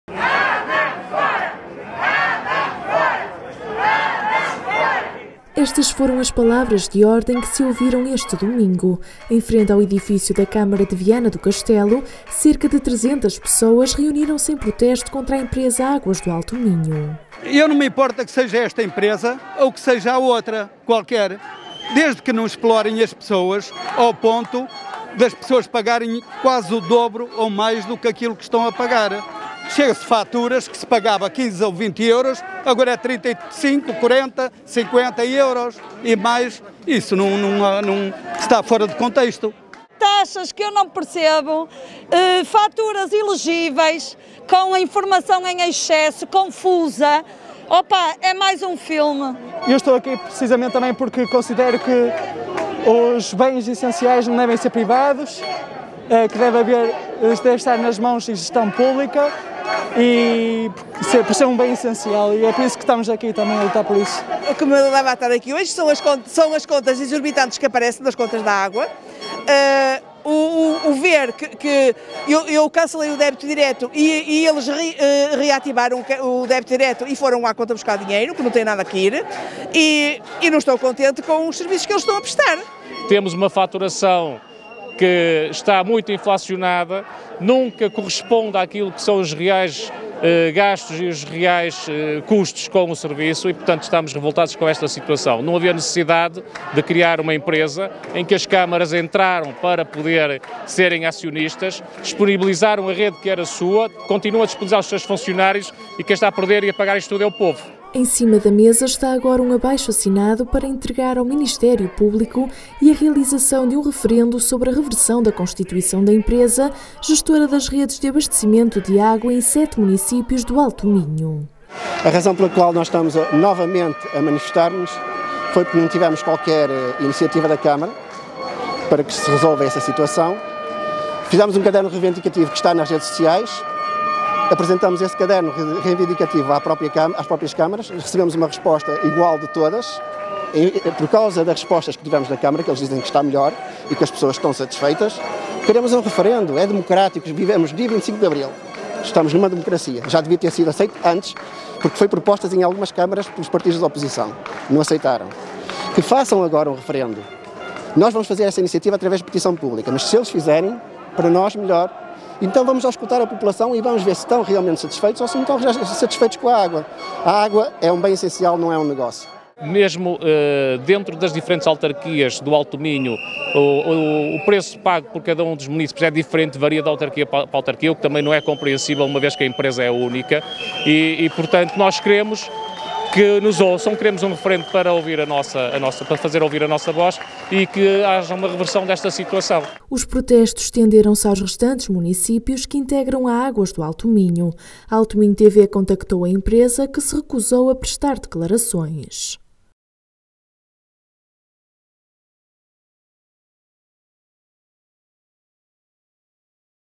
Cerca de 300 pessoas manifestam-se em Viana do Castelo contra Águas do Alto Minho | Altominho TV
Um movimento cívico criado nas redes sociais contesta a Águas do Alto Minho e defende a realização de um referendo sobre a reversão da constituição da empresa da água, a operar em sete municípios do Alto Minho. Consumidores reuniram-se em protesto em Viana do Castelo e nos restantes concelhos.